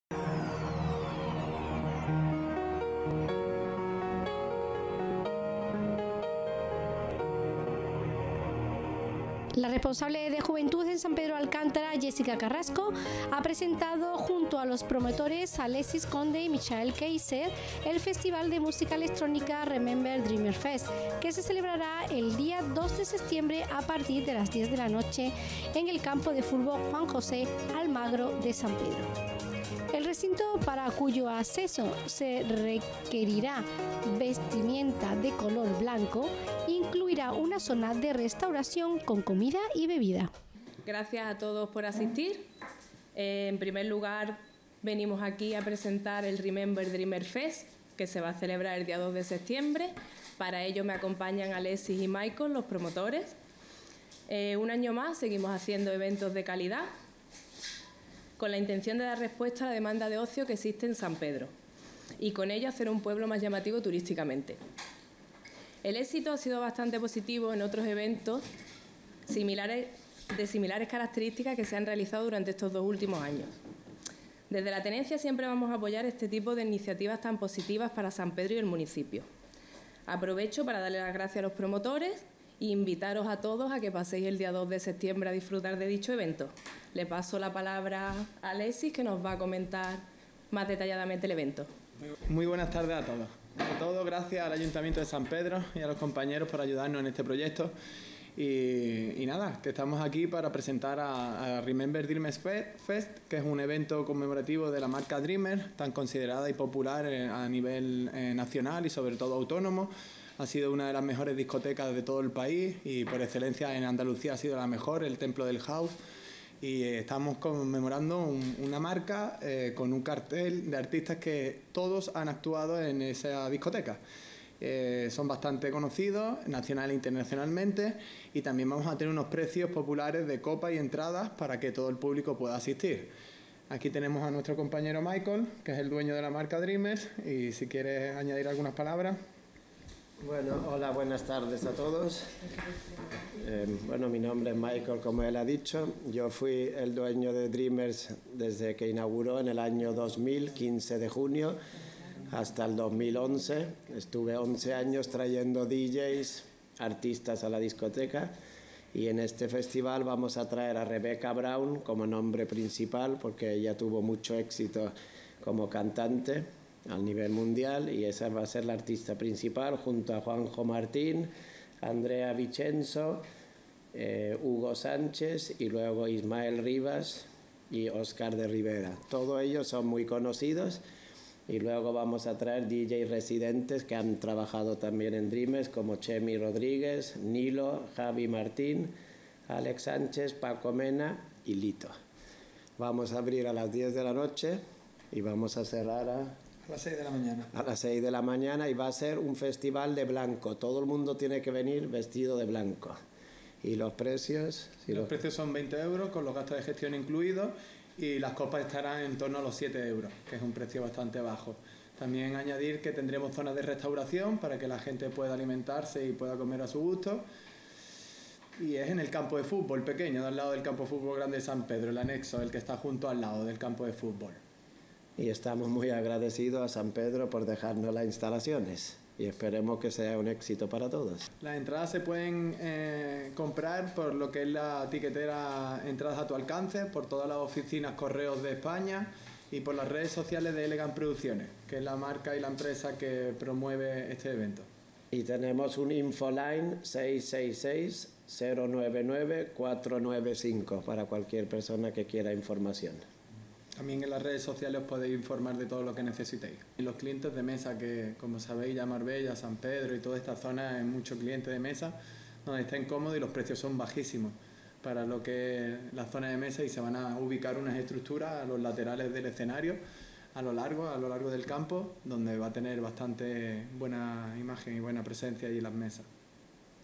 Rueda de Prensa Presentación del festival ‘Remember Dreamers Fest’
La responsable de Juventud en San Pedro Alcántara, Jessica Carrasco, ha presentado hoy, junto a los promotores Alexis Conde y Michael Keijzer, el festival de música electrónica ‘Remember Dreamers Fest’, que se celebrará el 2 de septiembre, a partir de las 22.00 horas en el Campo de Fútbol ‘Juan José Almagro’ de San Pedro.

 El evento, organizado en recuerdo de la discoteca Dreamers de Puerto Banús, tiene como cabeza de cartel a Rebeka Brown e incluye la participación de una decena de pinchadiscos, entre los que se encuentran ‘The Mix de Rivera&Rivas’, Juanjo Martín, André Vicenzzo, Hugo Sánchez y los residentes Txemy Rodríguez, Nilo, Javi Martín, Álex Sánchez, Paco Mena y Litto.

 El recinto, para cuyo acceso se requerirá vestimenta de color blanco, incluirá una zona de restauración con comida y bebida.

Las entradas, a partir de 20 euros por persona, se pueden adquirir a través de la página web de Kingocio.